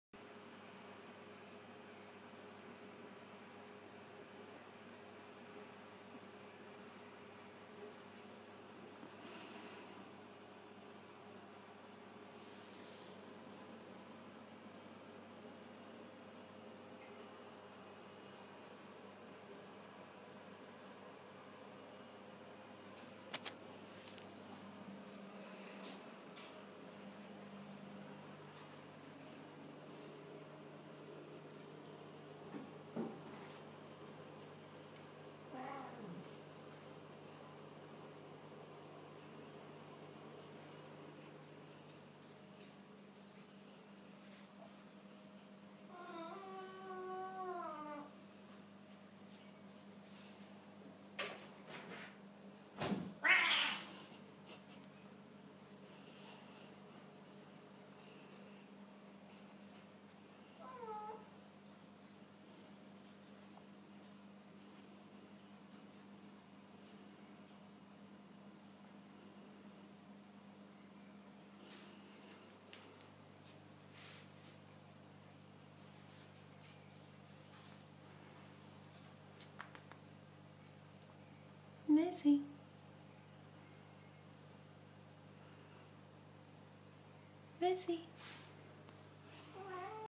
Cute Black Kitten Stalks Angry Siamese
Cute playful little black kitten stalks angry Siamese. Cat fight!!!